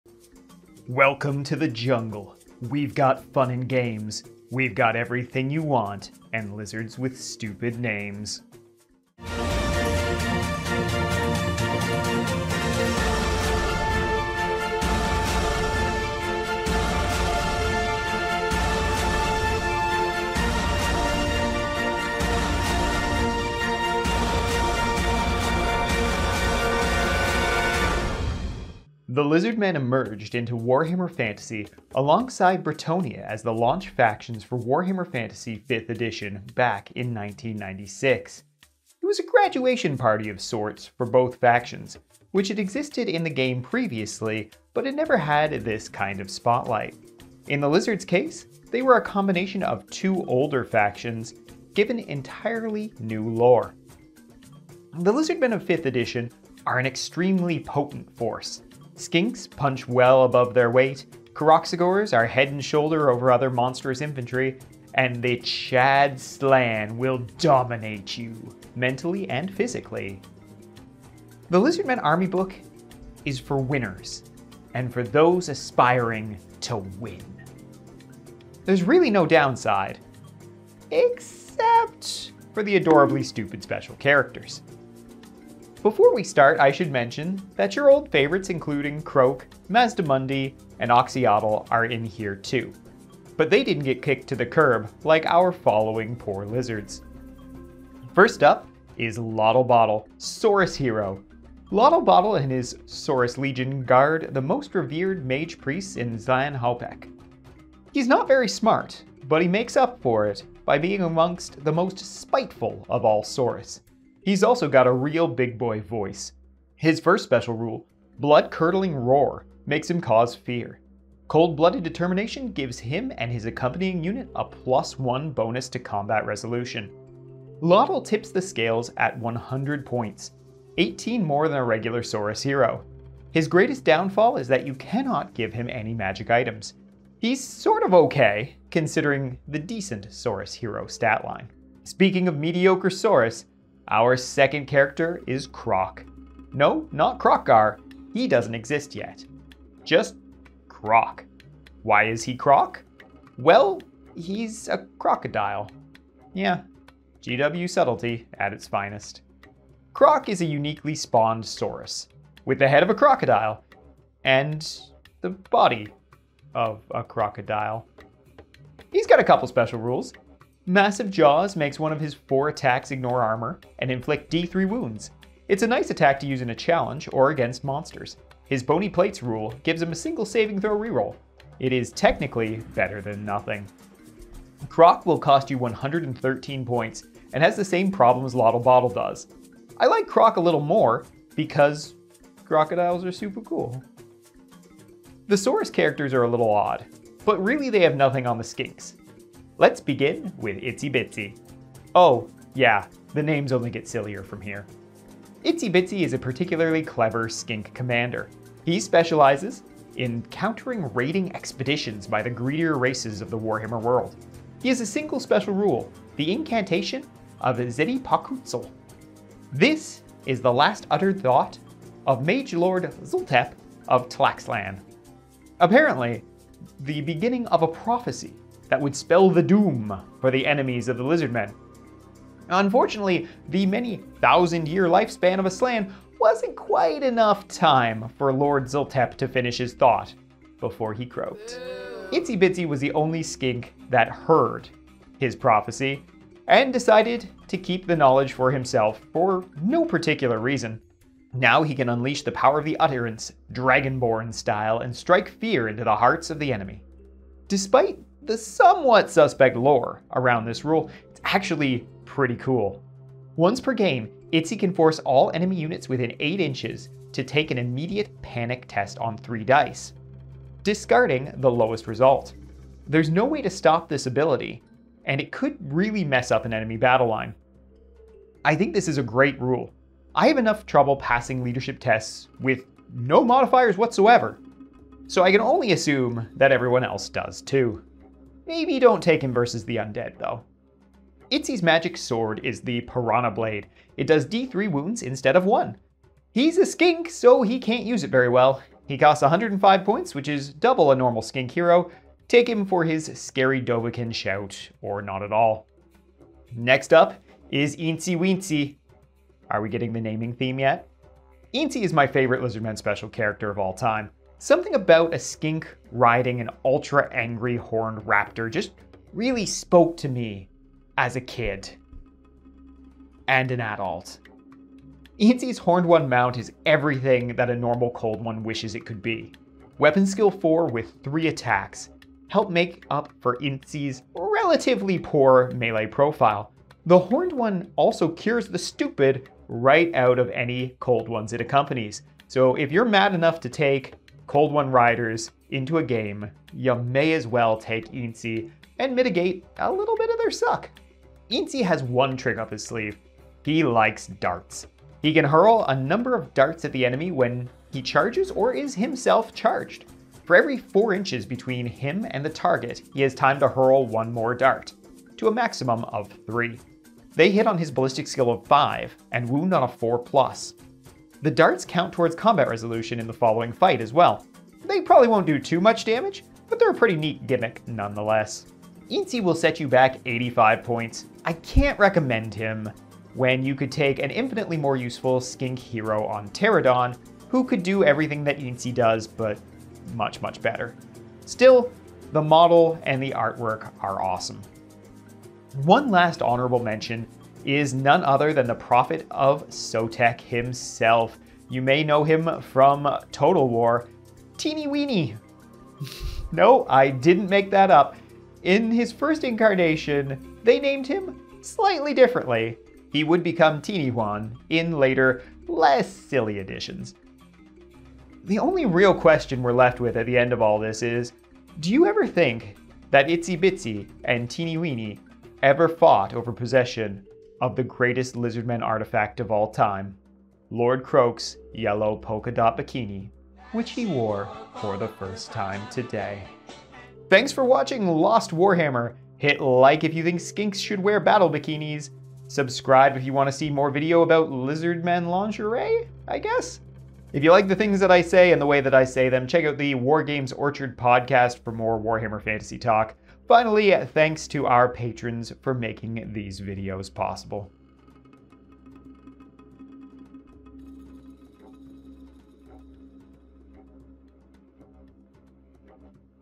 Lizardmen gone wild! The wackiest special characters in Warhammer Fantasy. Lost Warhammer #1
Seems GW had a little too much fun designing the Lizardmen Special Characters in 5th edition. Join us for a look at these one-and-done Characters who were to silly to survive past the 90's. Featuring some of the best / worst names in Warhammer History!

Help us out and get extra stuff